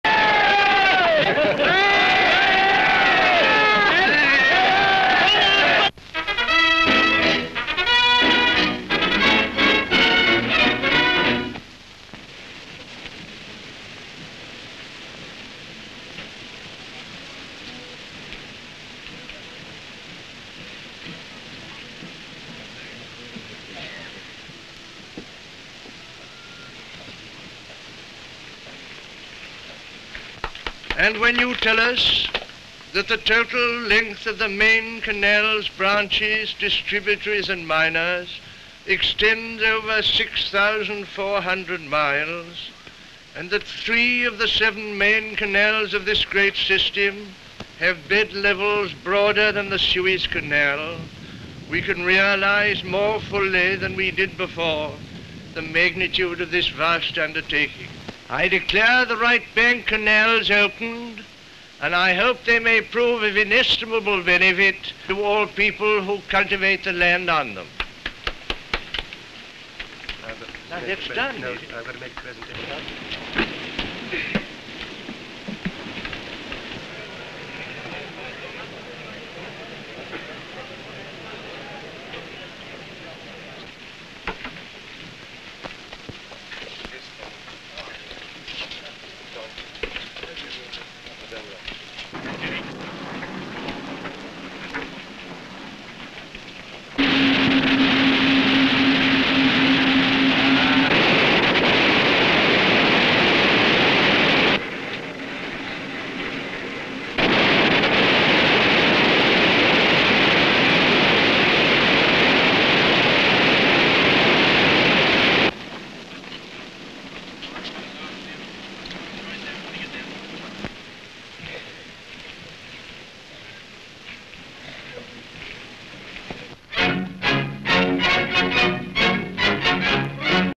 Lloyd Barrage Has Viceregal Opening
(11 Feb 1932) First sound pictures of the ceremony at Sukkur, when Lord Willingdon releases great irrigation scheme.